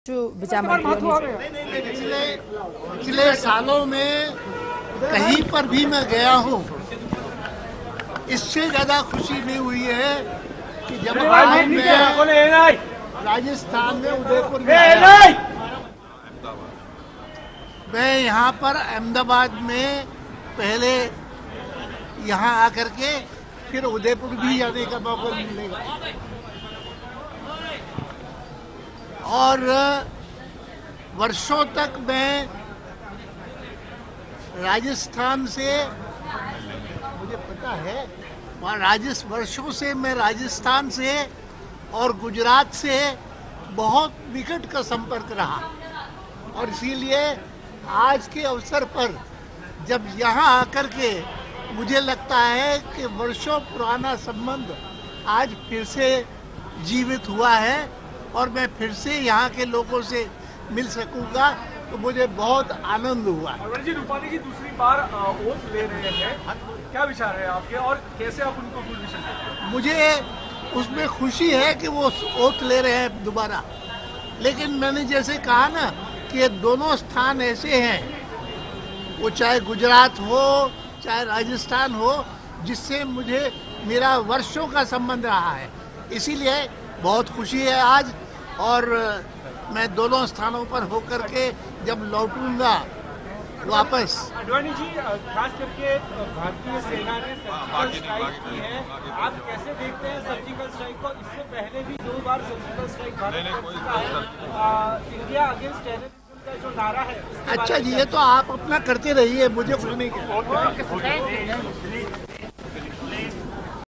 LK Advani congratulates Vijay Rupani on taking oath as CM for second time | Vtv News
LK Advani congratulates Vijay Rupani on taking oath as CM for second time